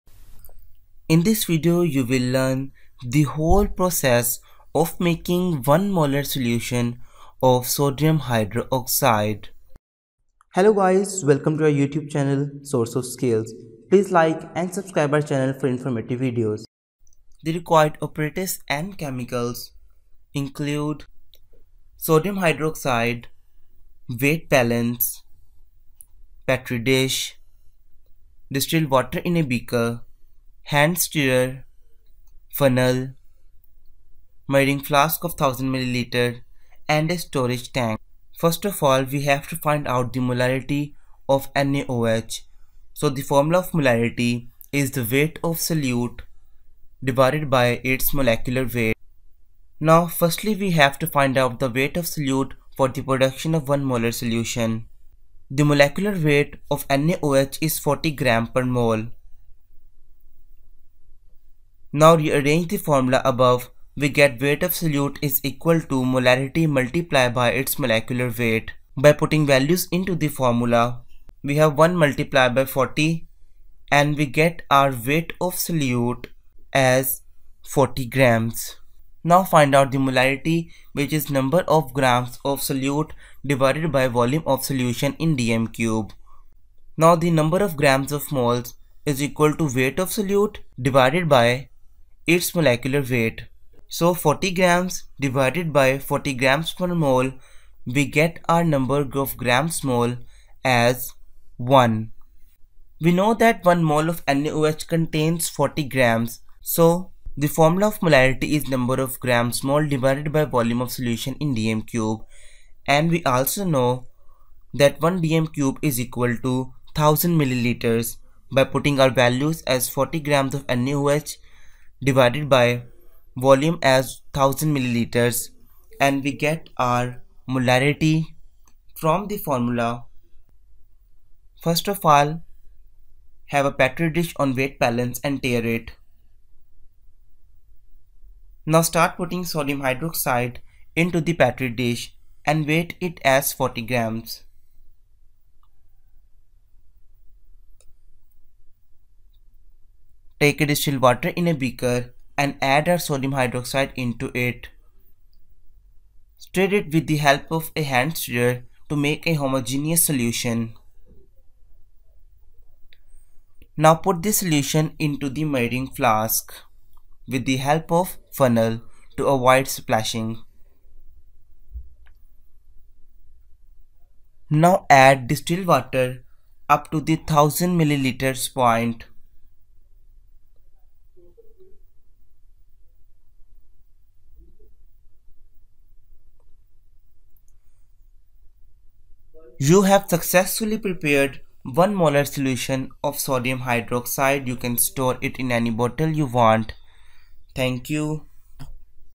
1 molar solution of NaOH | 1 m NaoH | 1 molar NaOH calculation
by this animation you will learn how to prepare 1 molar solution of NaOH.